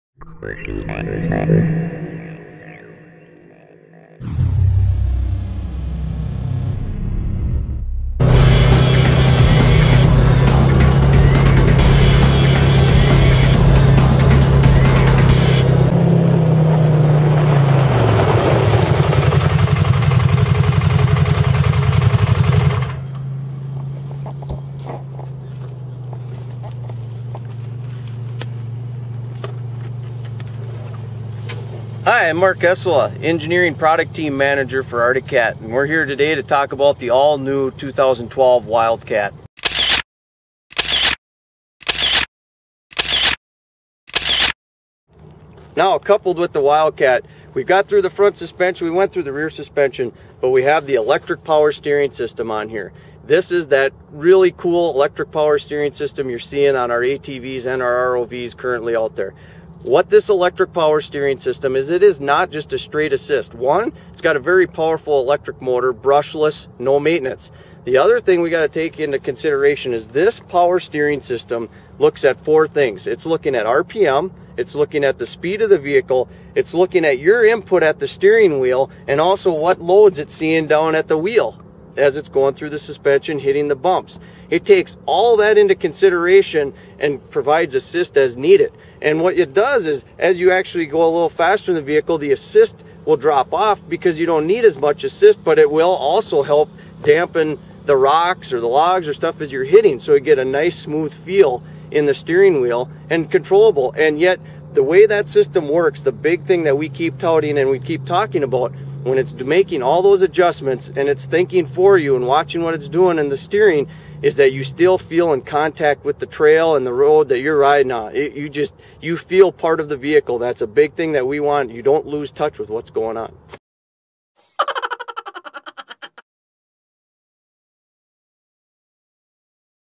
Arctic Cat Wildcat Interview 5 UTV W - Wildcat Power steering by Arctic CAt Side by Sides
- The Arctic CAt Wildcat Side by Side has amazing reactive power steering that load adjusts according to the speed of the Wildcat UTV on the trail.  Go Arctic Cat Side by Sides!